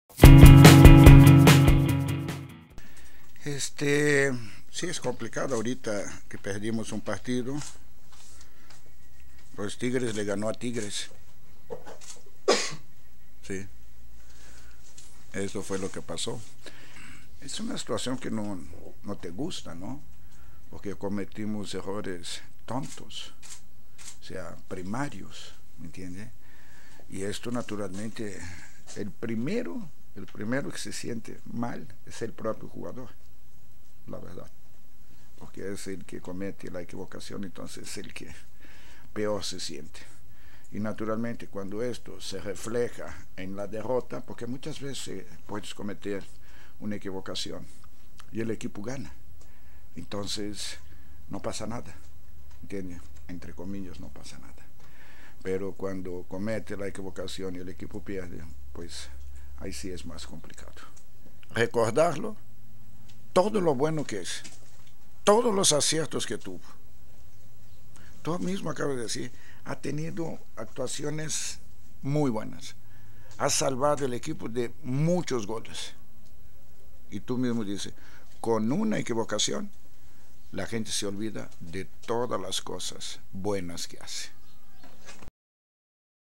Tigres le ganó a Tigres: Ricardo Ferretti
Para el Tuca, su equipo cayó debido a los errores que se cometieron más que a los aciertos del rival